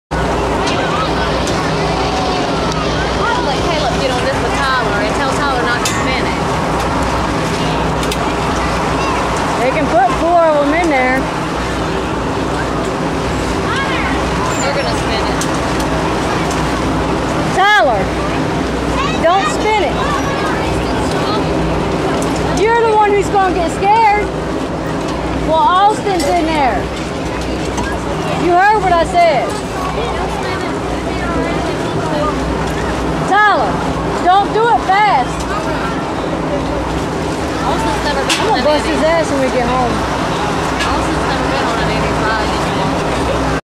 Riverblast 2009 Part 8
Independence Day Celebration at the Riverblast 2009 in Augusta Georgia